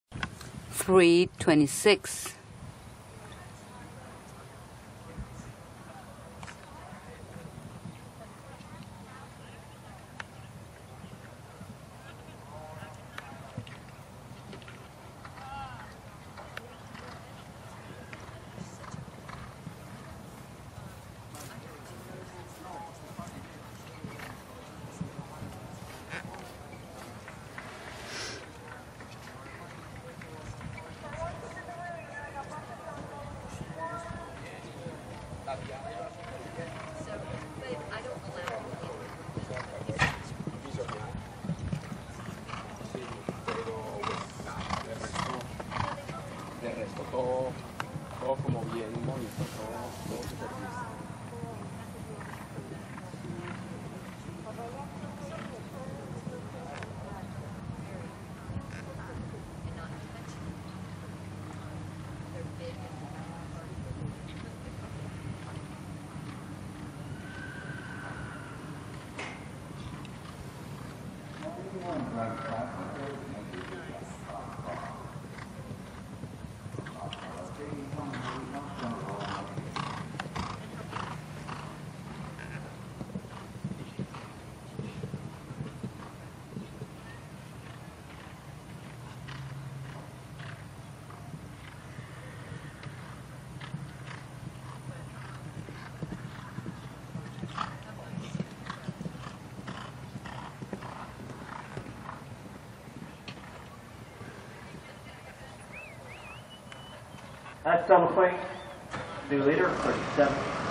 Video of Chiquita ridden by Jordyn Friedman from ShowNet!
Chiquita ridden by Jordyn Friedman at Ocala I 2016.  This round is from class 216, 1.15m Back on Track Jumper.